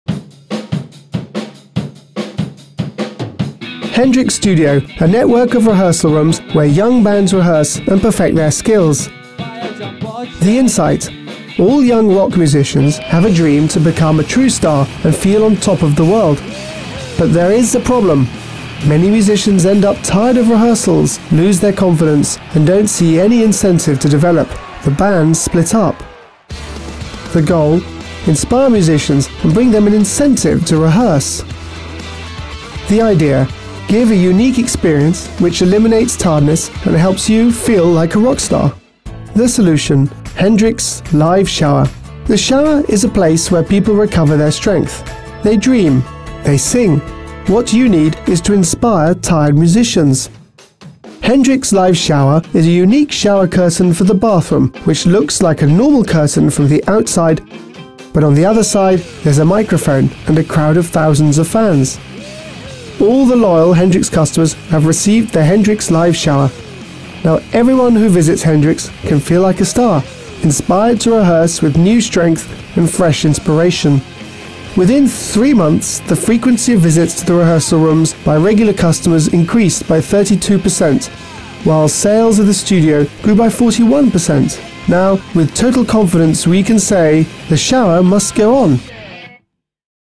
Live shower
Problem. Musicians are tired and disincentivised to rehears over and over again. The bands split up.Goal To inspire, restore power.Insight. Their dream is to become a rock star.Idea.  Inspiring brand experience at the right time cure for tiredness  dream renewed.Solution. HendrixLiveShower - unique bathroom curtain. Bathroom is a place where people sing. We created a curtain with a microphone and a crowd of thousands fans on it. Loyal HendrixStudio customers have received the HendrixLiveShower after their rehearsals.